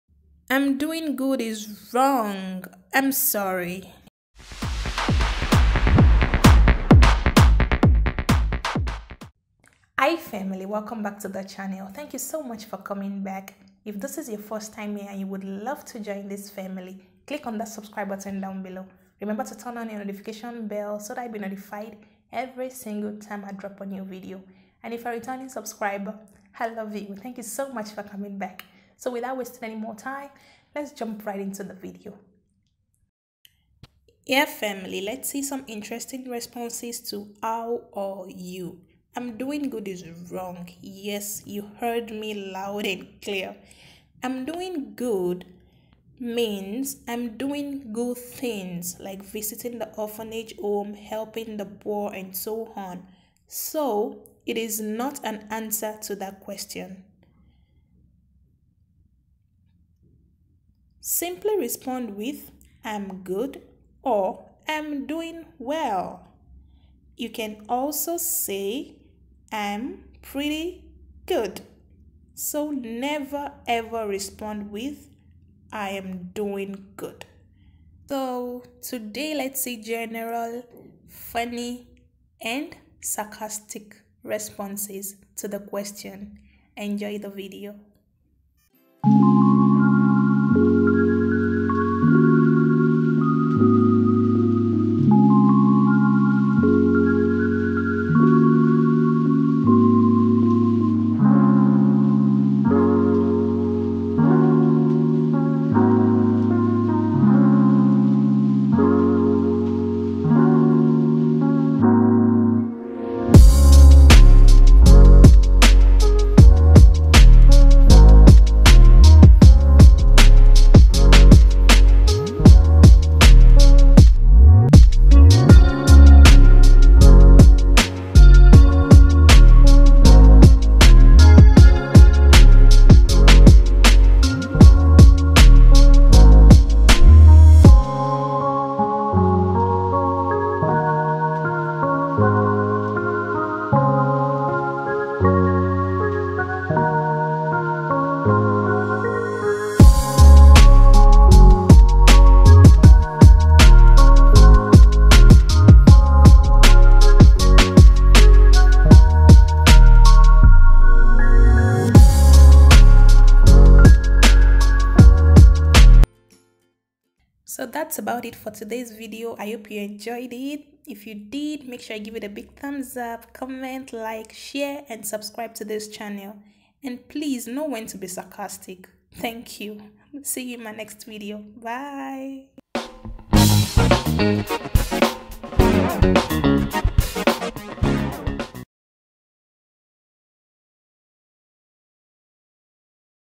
Interesting ways to respond to "HOW ARE YOU?"
Hi Family, welcome back!
"How are you?" is a question we all get everyday and doesn't it feel like we are stuck with just one major reply?

In this video, there are some interesting responses to spice up the reply a little more. Enjoy the video ❤️